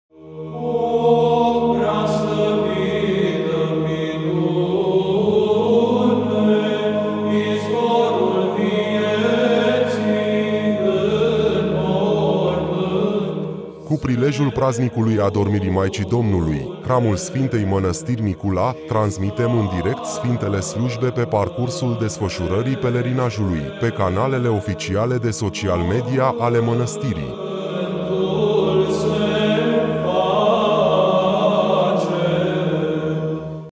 Spot Hram 2023
Cu prilejul praznicului Adormirii Maicii Domnului, hramul Sfintei Mănăstiri Nicula, transmitem în direct sfintele slujbe pe parcursul desfășurării pelerinajului, pe canalele oficiale de social media ale mănăstirii.

🔔 Nu uitați să vă abonați la canal și să distribuiți.